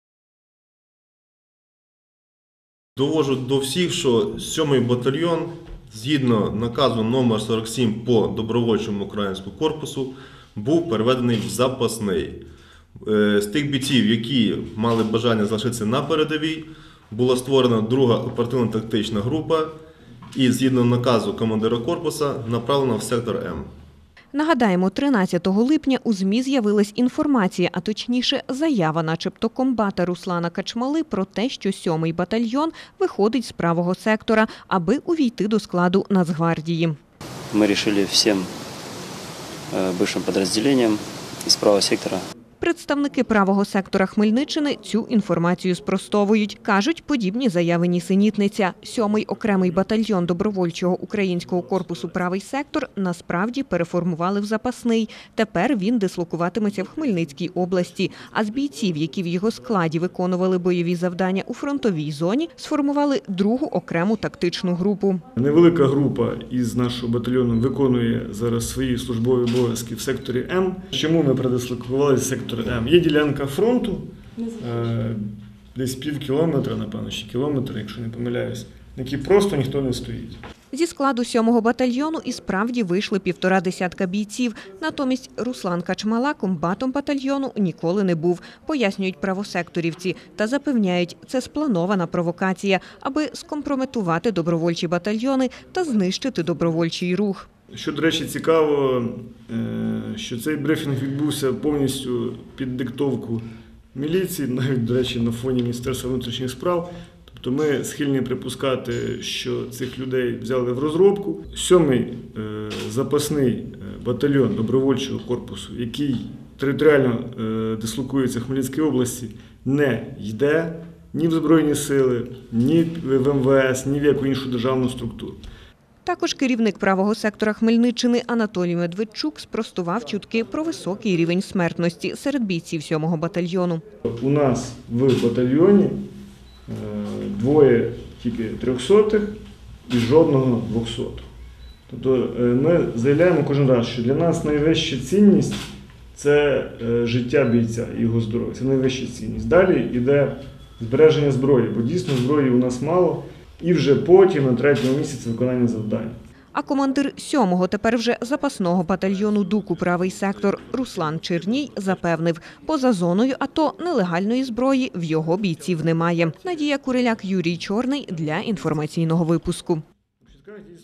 ХОДТРК "Поділля-центр" - Правий сектор
Правий сектор знову в центрі уваги. День тому в медіа з’явилась інформація про те,   що 7-й батальйон Добровольчого українського корпусу “Правий Сектор”   увійде до складу Національної гвардії. Подібні заяви спростовують представники Хмельницького осередку організації та й сам командир сімки Руслан Черній.